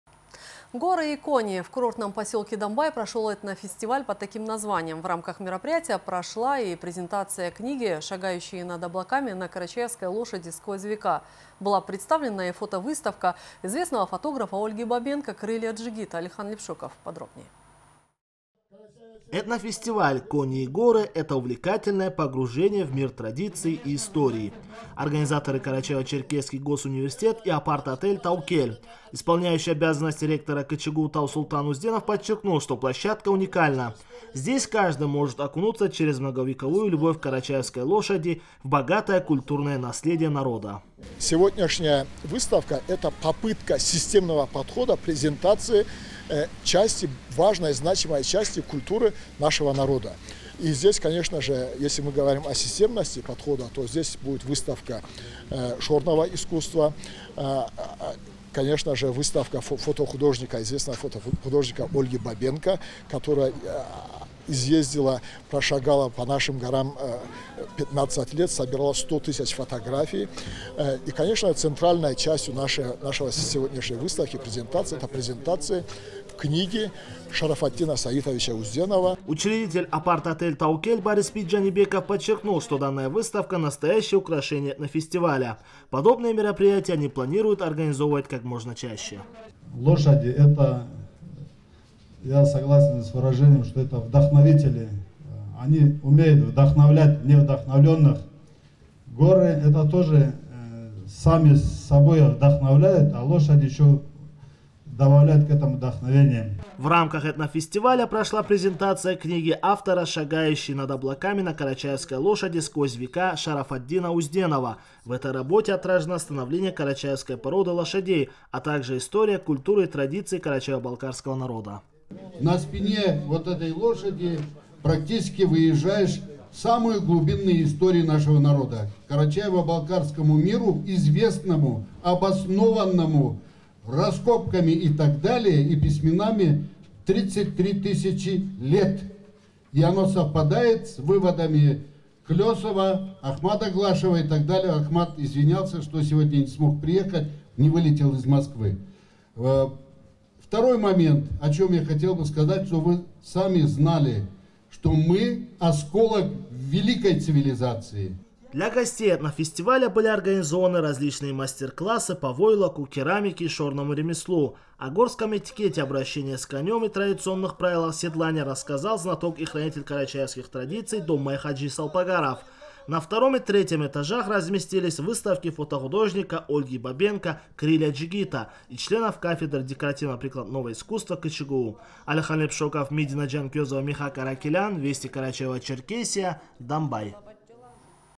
В курортном поселке Домбай прошел этнофестиваль «Горы и кони»
«Горы и кони» — в курортном поселке Домбай прошёл этнофестиваль под таким названием. В рамках мероприятия прошла и презентация книги «Шагающие над облаками. Карачаевские лошади сквозь века». Была представлена и фотовыставка известного фотографа Ольги Бабенко «Крылья джигита».